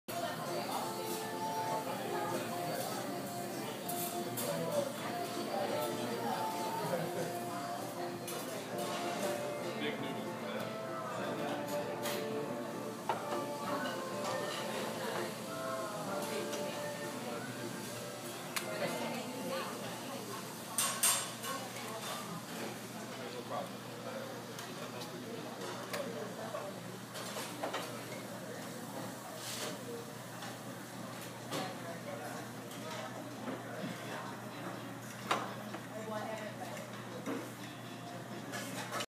Flaming Grill & Buffet Video 1 2 2014
The Flaming Grill & Buffet in Frederick, MD. Love the blue lights as well as the food.